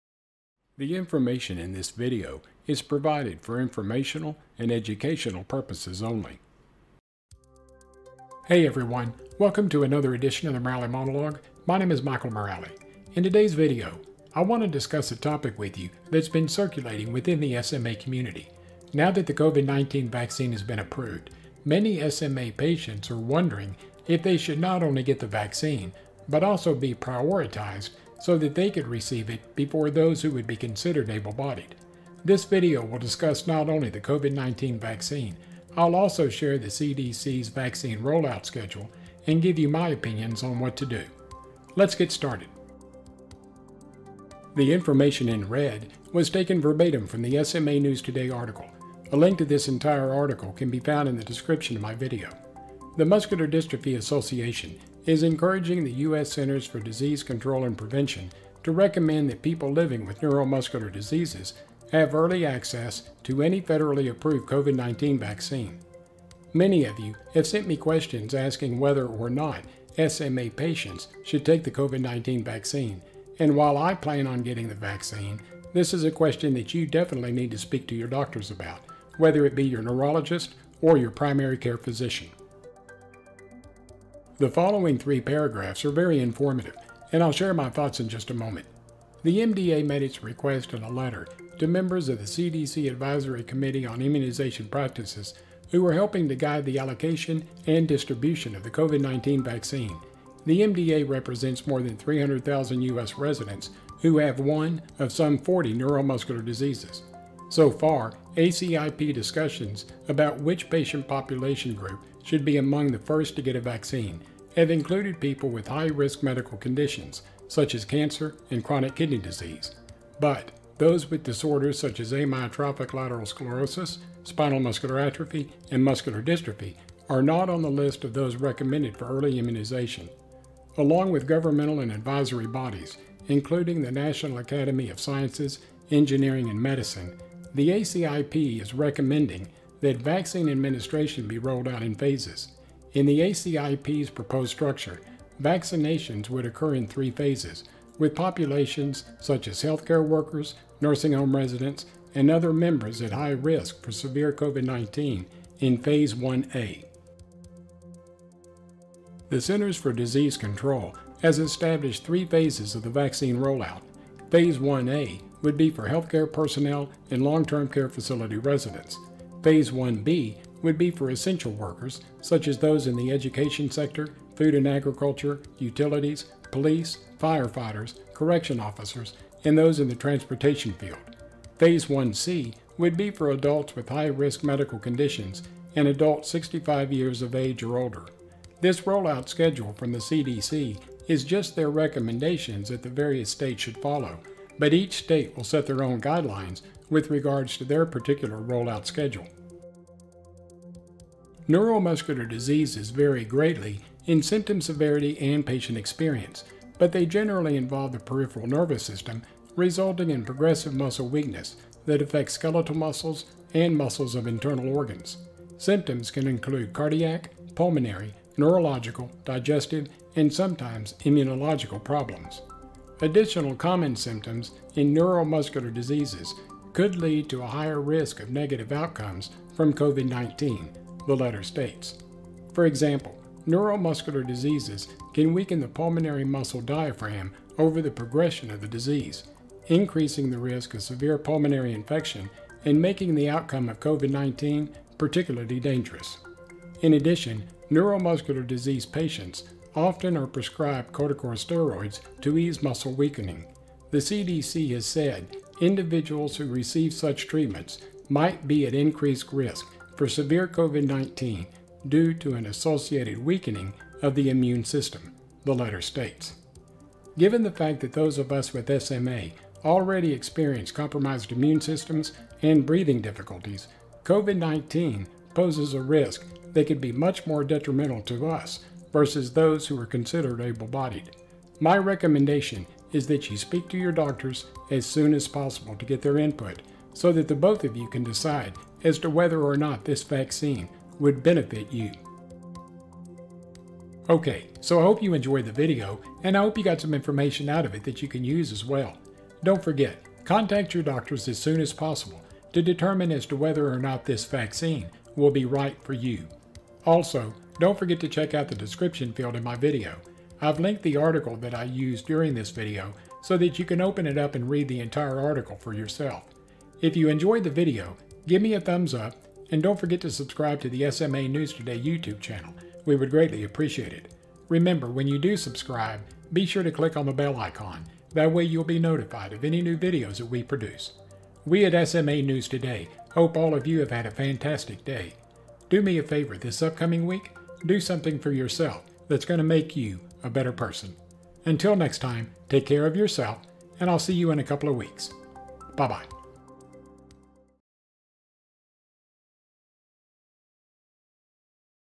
Should SMA Patients Take The COVID 19 Vaccine?
In this episode of the Morale Monologue, I discuss a question that’s been floating around the SMA community for over a month. Many of you have asked the question regarding whether or not patients with SMA should take the COVID-19 vaccine. In my video, I review an article that was recently published by SMA News Today, and this article goes into great detail regarding the vaccine and the rollout schedule that was developed by the Centers for Disease Control.

To take part in our ongoing discussions regarding SMA, please